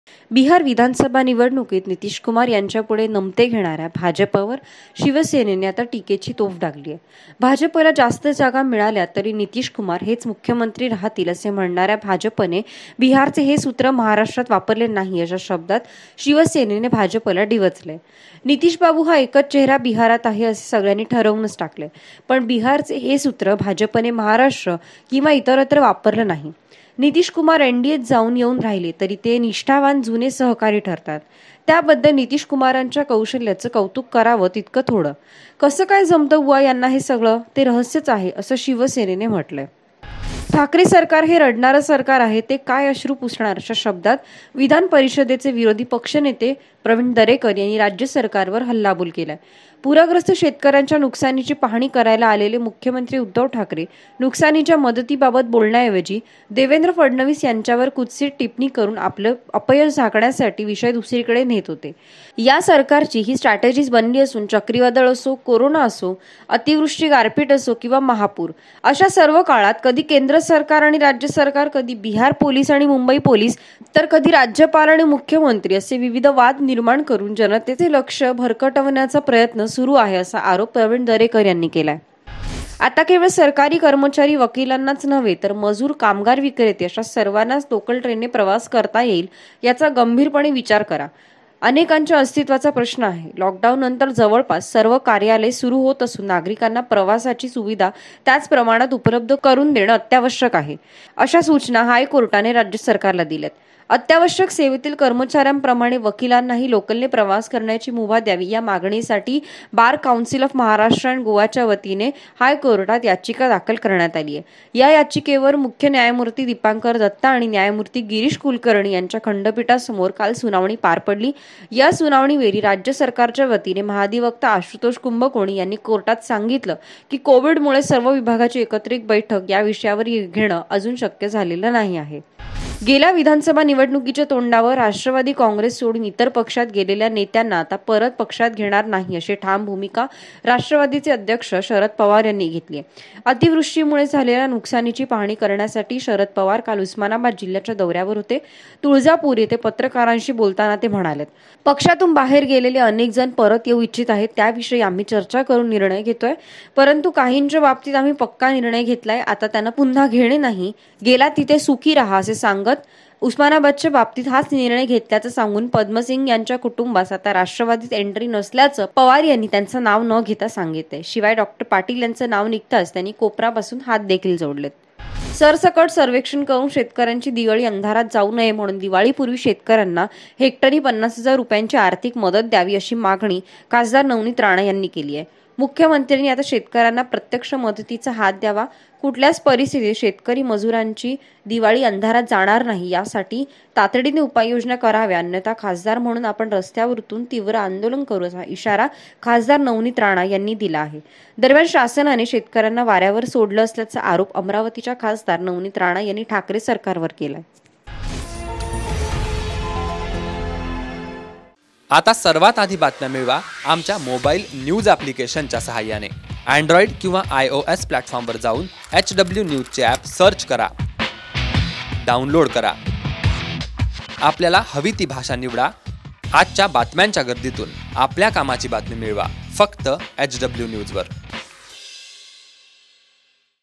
पक्ष सोडून गेलेल्यांना शरद पवारांचा ‘नो एंट्रीचा बोर्ड’ तर बिहारच्या सुत्रावर सामनातून टीका!
बिहार विधानसभा निवडणुकीत नितीशकुमार यांच्यापुढे नमते घेणाऱ्या भाजपवर शिवसेनेने टीकेची तोफ डागली आहे. भाजपला जास्त जागा मिळाल्या तरी नितीशकुमार हेच मुख्यमंत्री राहतील असे म्हणणाऱ्या भाजपने बिहारचे हेच सूत्र महाराष्ट्रात वापरले नाही,' अशा शब्दांत शिवसेनेने भाजपला डिवचले आहे.'नितीशबाबू हा एकच चेहरा बिहारात आहे, असे सगळ्यांनी ठरवूनच टाकले आहे. पण बिहारचे हे सूत्र भाजपने महाराष्ट्र किंवा इतरत्र वापरले नाही. नितीशकुमार ‘एनडीए’त जाऊन येऊन राहिले. तरी ते निष्ठावान, जुने सहकारी ठरतात. त्याबद्दल नितीशकुमारांच्या कौशल्याचे कौतुक करावे तेवढे थोडेच. कसे काय जमते बुवा यांना हे सगळे? ते रहस्यच आहे,' असं शिवसेनेनं म्हटलं आहे.गेल्या विधानसभा निवडणुकीच्या तोंडावर राष्ट्रवादी काँग्रेस सोडून इतर पक्षात गेलेल्या नेत्यांना आता परत पक्षात घेणार नाही, अशी ठाम भूमिका राष्ट्रवादीचे अध्यक्ष खासदार शरद पवार यांनी घेतली आहे.अतिवृष्टीमुळे झालेल्या नुकसानीची पाहाणी करण्यासाठी खा. पवार सोमवारी उस्मानाबाद जिल्ह्याच्या दौºयावर होते. तुळजापूर येथे पत्रकारांशी बोलताना ते म्हणाले, पक्षातून बाहेर गेलेले अनेकजण परत येऊ इच्छित आहे. त्याविषयी आम्ही चर्चा करून निर्णय घेतोय. परंतु, काहींच्या बाबतीत आम्ही पक्का निर्णय घेतलाय. आता त्यांना पुन्हा घेणे नाही. ‘गेलात तिथे सुखी रहा’, असे सांगतोय़ उस्मानाबादच्या बाबतीत हाच निर्णय घेतल्याचे सांगून पद्मसिंह यांच्या कुटूंबास आता राष्ट्रवादीत एन्ट्री नसल्याचे पवार यांनी त्यांचे नाव न घेता सांगितले. शिवाय डॉ. पाटील यांचे नाव निघताच त्यांनी कोपरापासून हात जोडले!